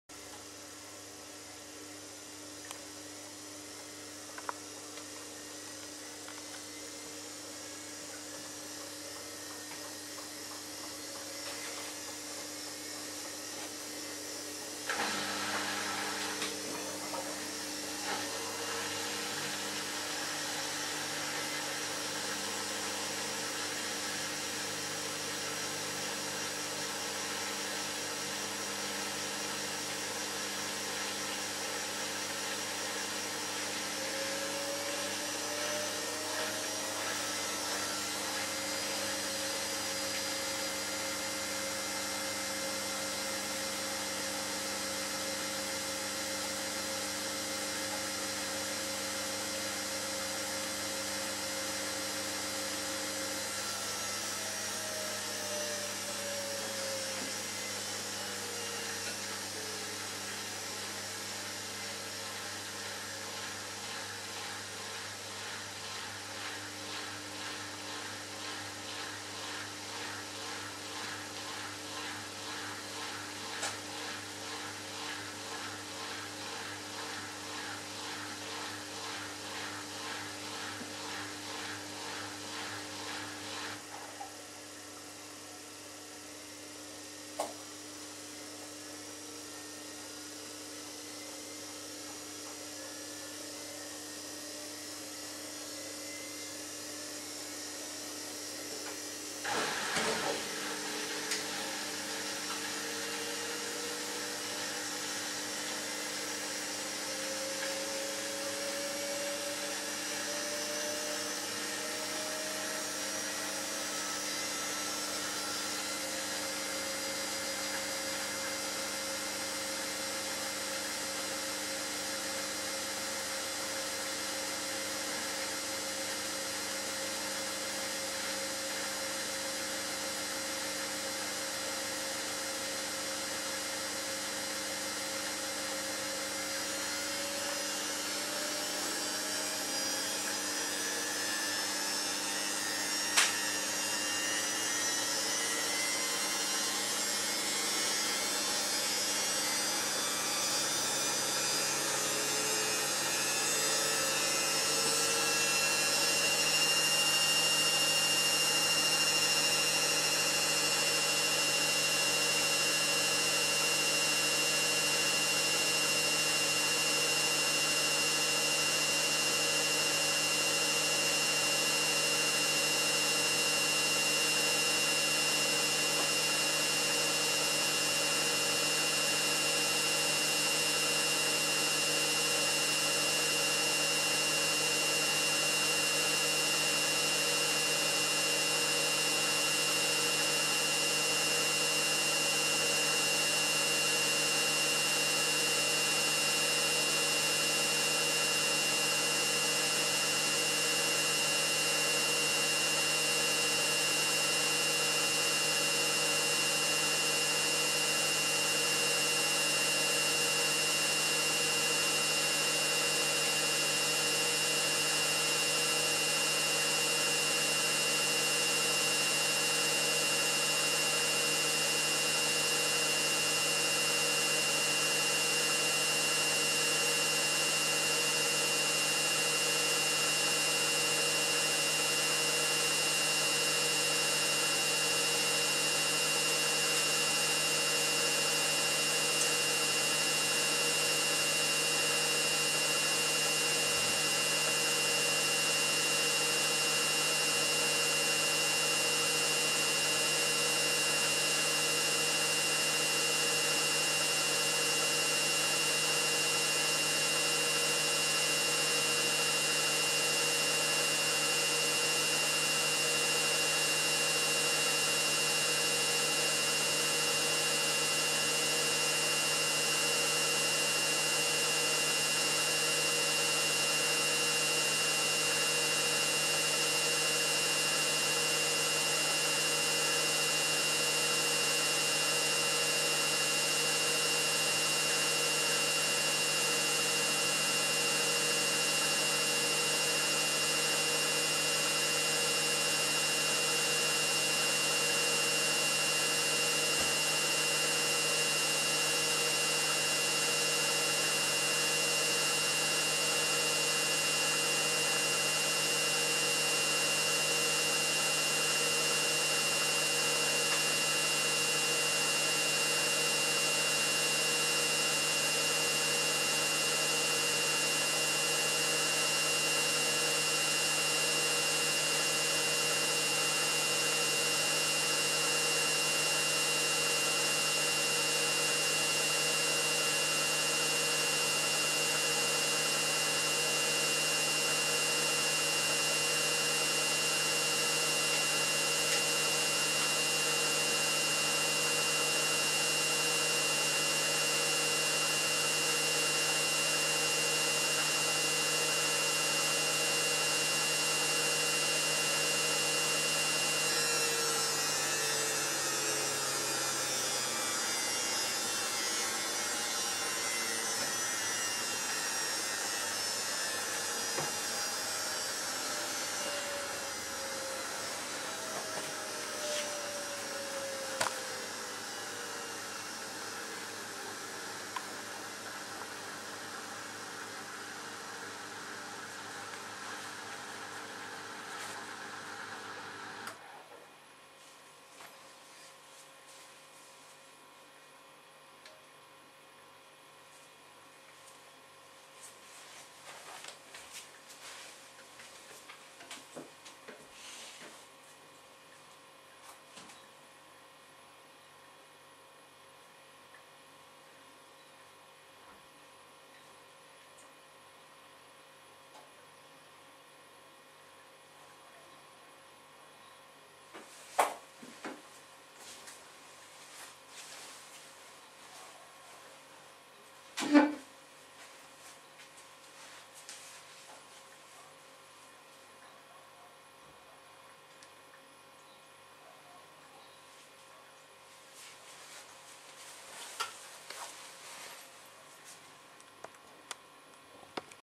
Samsung Ecobubble, Quick Wash 50 minutes cycle 4/4
In this video:
Continuation of the final spin